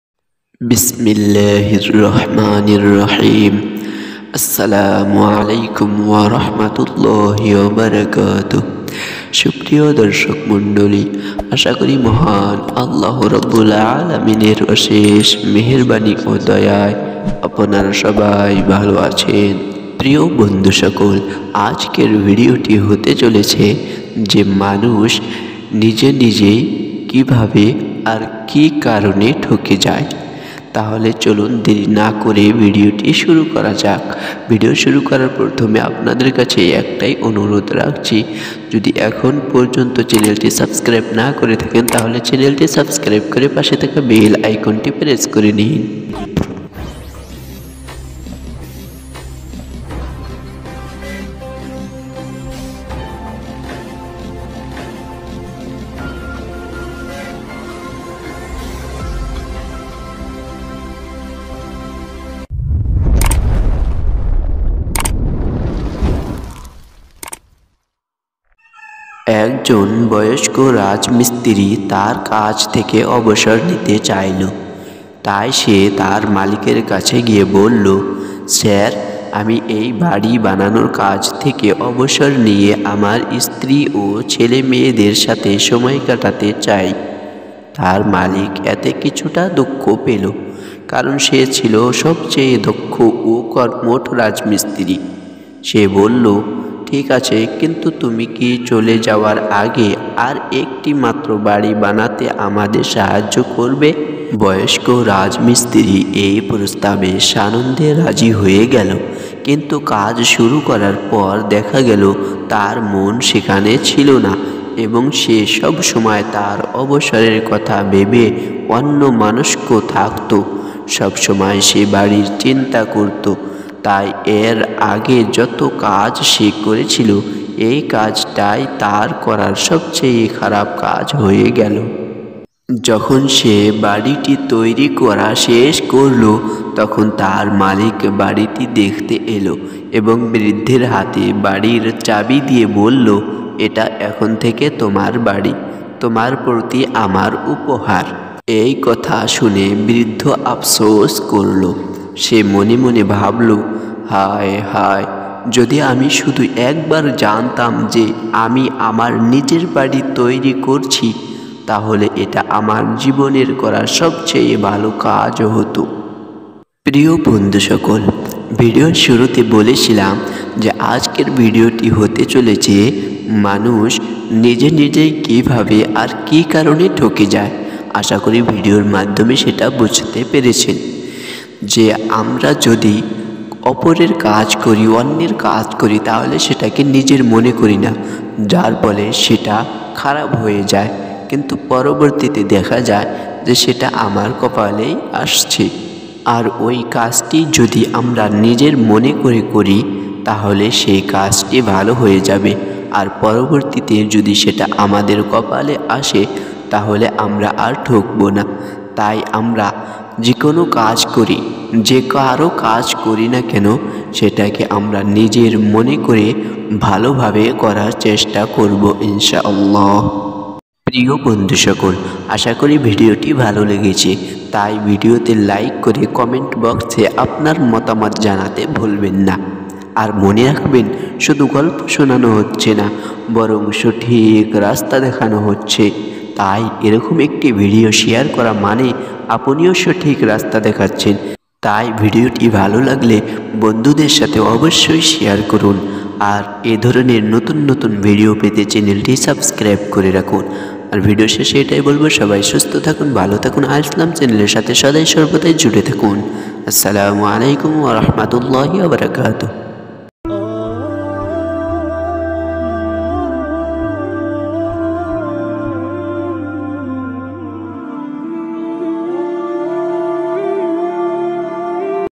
মানুষ কিভাবে আর কি কারণে নিজে নিজেই ঠকে? | একটি জীবন বদলানো শিক্ষামূলক গল্প | R Islam SnT.
About This Video:
   প্রিয় বন্ধু সকল! আজকের এই ভিডিওতে দেখানো হয়ছে যে মানুষ নিজে নিজে কিভাবে আর কি কারনে ঠকে? আর যে কারো কাজ যেকোনো কাজ করার সময় আমাদের কি করা উচিত আর কি ভাবা উচিত?। একটি শিক্ষামূলক গল্প সাথে।

     Qures Solve:
১/যেকারো কাজ যেকোনো কাজ করার সময় আমাদের কি ভাবা উচিত?
২/মানুষ নিজে নিজে কিভাবে আর কি কারনে ঠকে?
৩/একটি জীবন বদলানো গল্প।
৪/একটি শিক্ষামূলক গল্প।